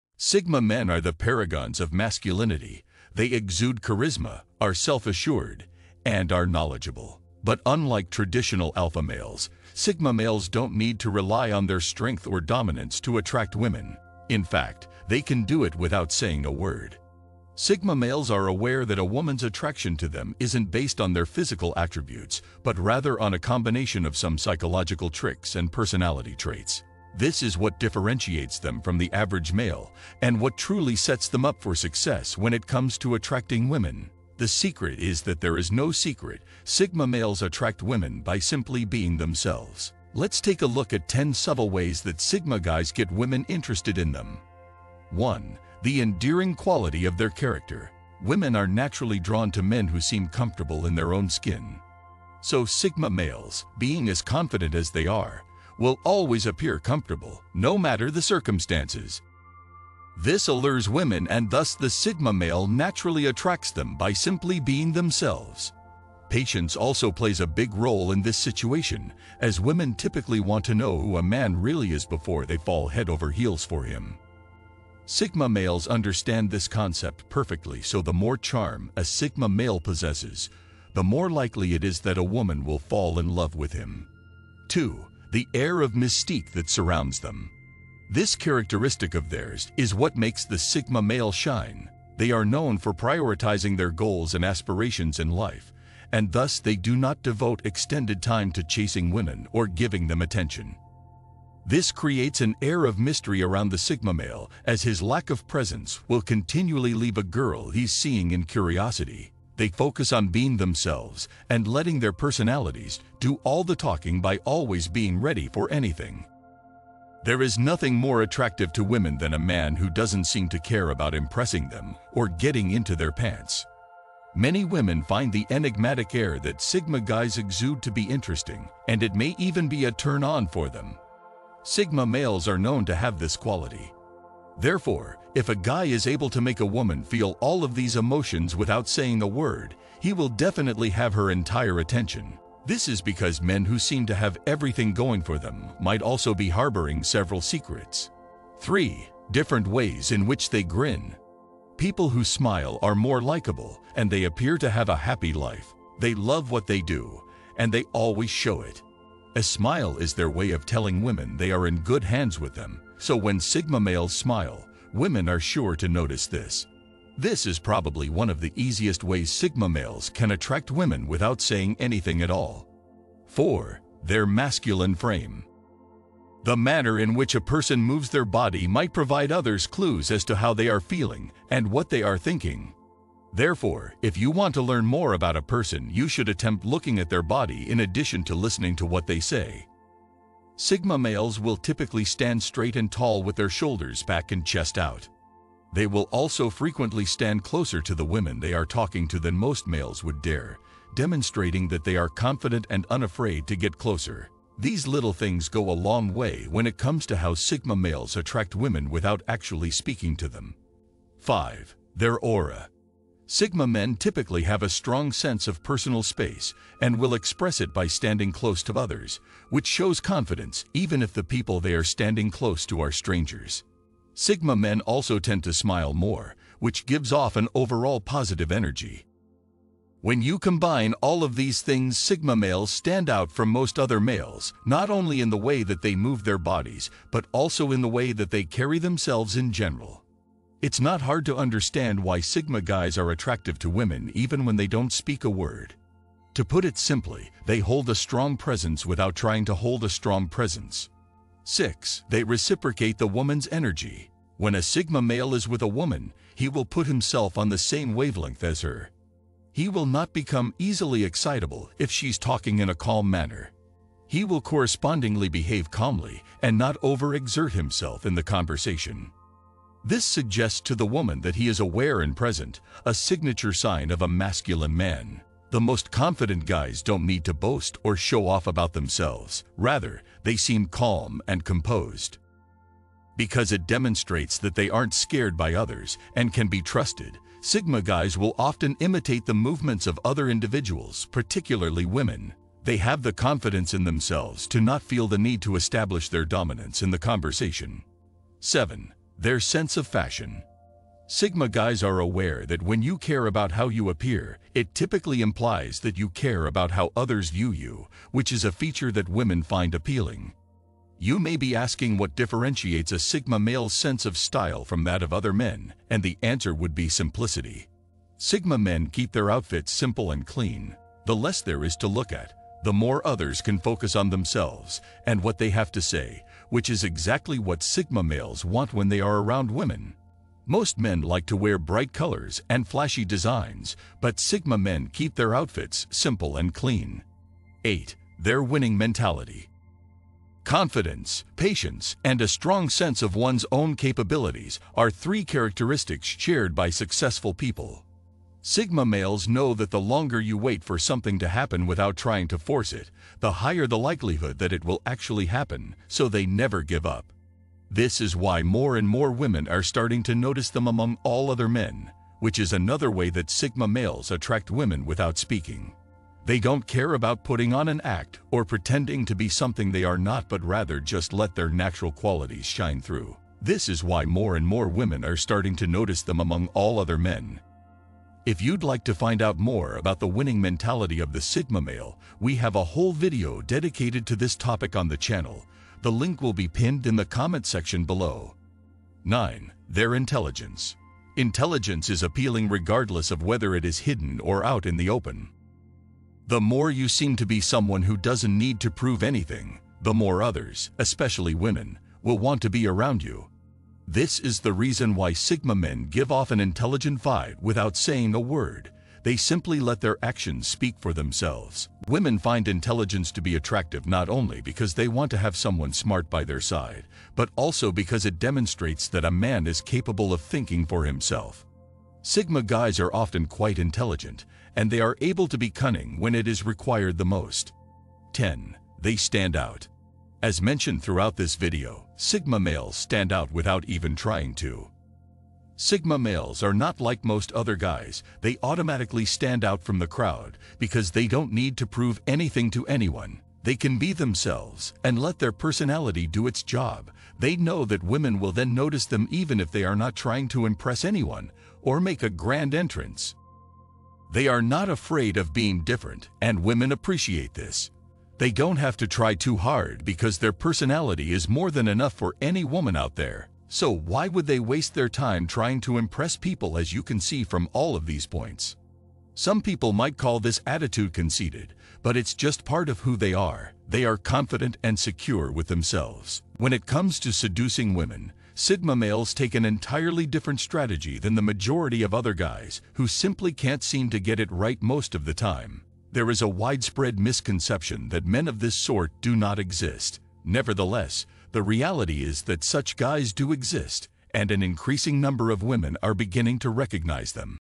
10 SECRET Ways Sigma Males Attract Women
In this video, I'm going to show you 10 SECRET Ways Sigma Males Attract Women. These tips will help you gain the attention of the women you want, and attract the women you want in your life.

There are a lot of things you can do to improve your dating life, and this video is a resource to help you get started. Whether you're looking for tips on attracting women in general, or tips on attracting women who are interested in Sigma males, this video is for you!